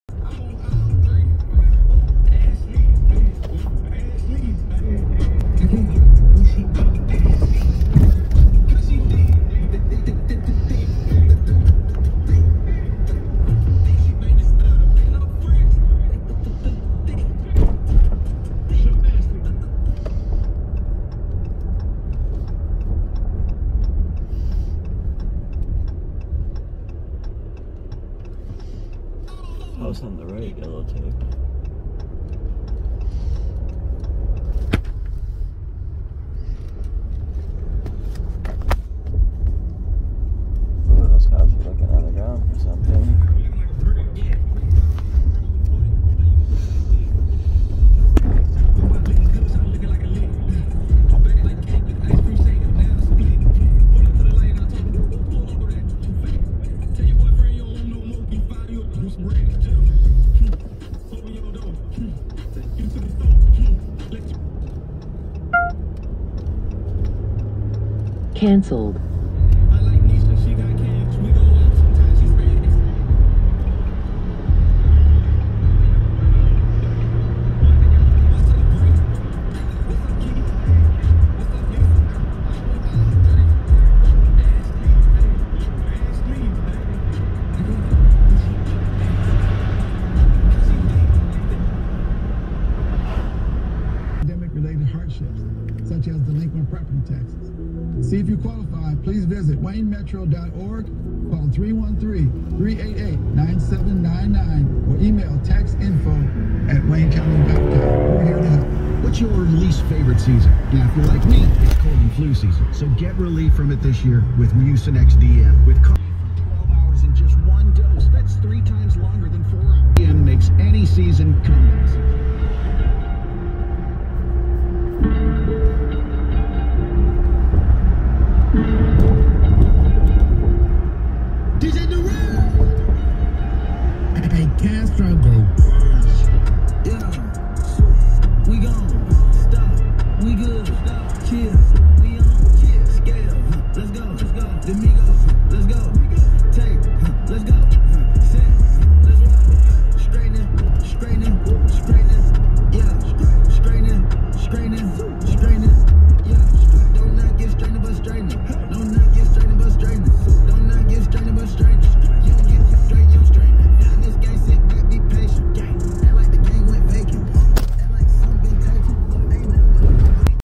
Detroit Michigan police At possible murder residents possibly looking for gun on the ground police
Detroit Michigan Saturday night police at what looks to appear to be a murder scene possibly looking for a gun on the ground with a house taped off at caution tape